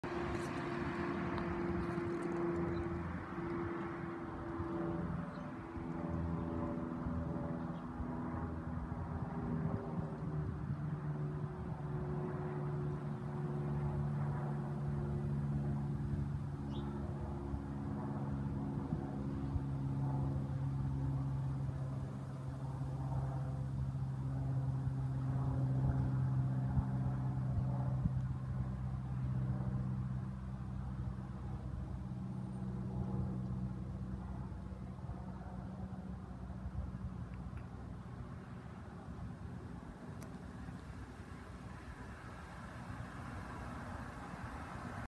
Cessna 172 arrival
Taken in my neighborhood while on a walk. If you listen closely, you can hear the Cessna's throttle being lowered.